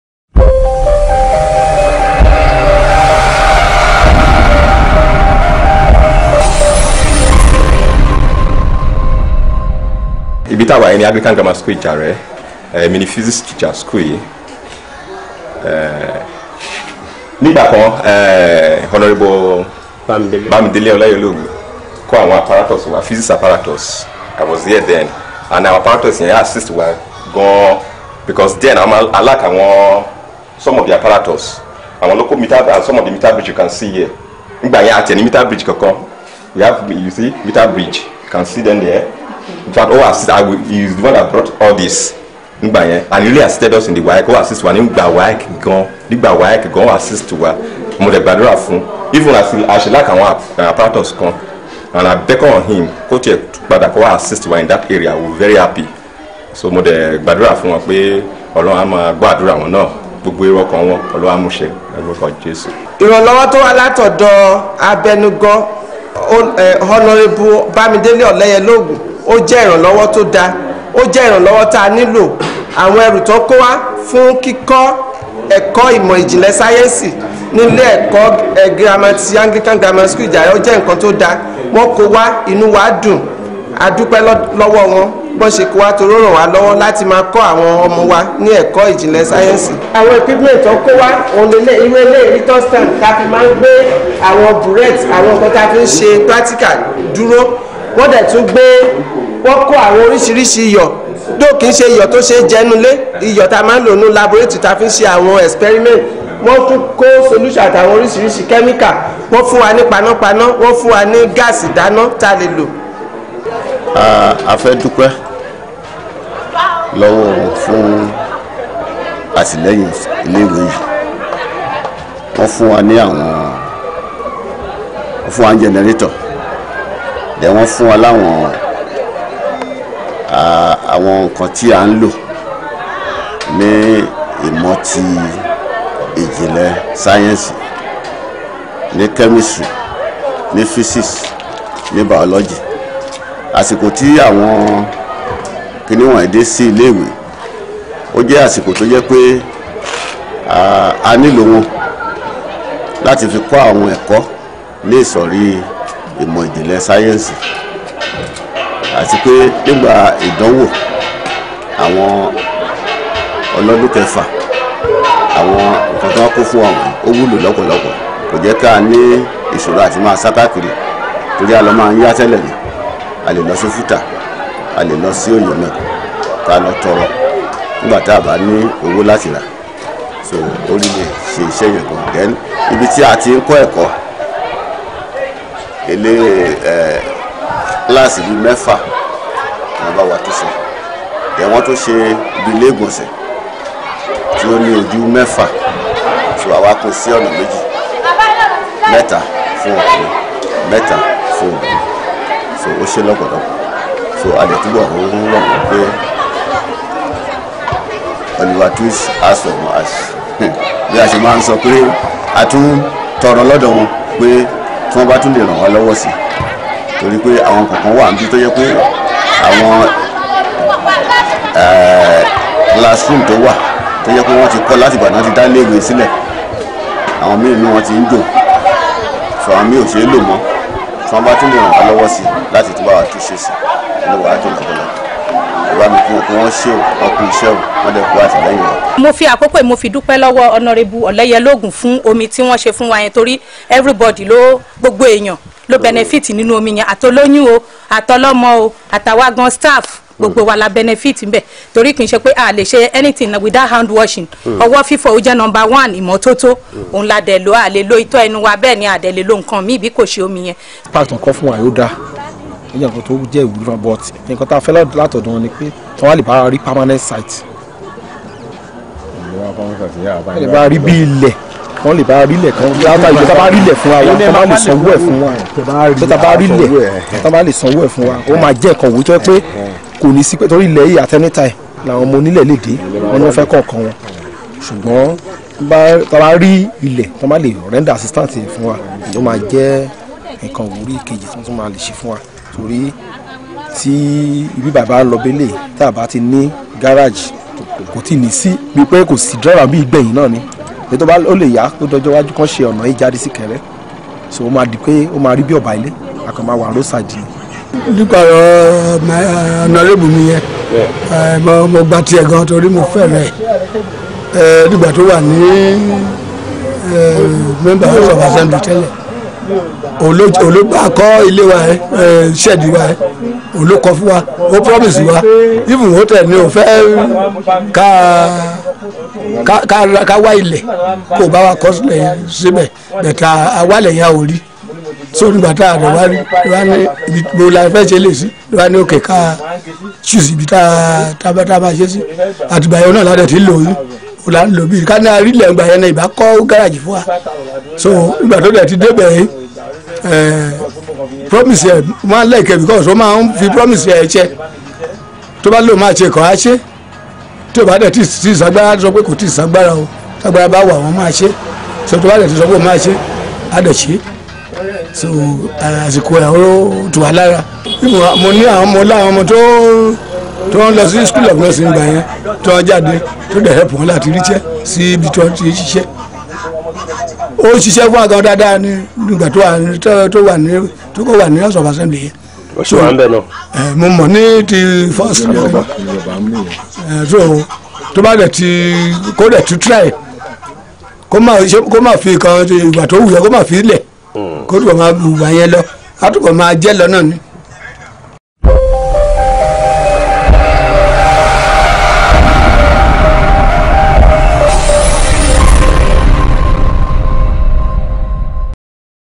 Video: Itesiwaju 19: Ohun Ti Awon Ara Ilu So Nipa Rt Hon. Bamidele Oleyelogun | The Precision TV
In this interview conducted in Yoruba language, residents of Ifedore local government in Ondo state appraised the contributions of the speaker of the state house of assembly, Rt Hon. Bamidele Oleyelogun to the council area.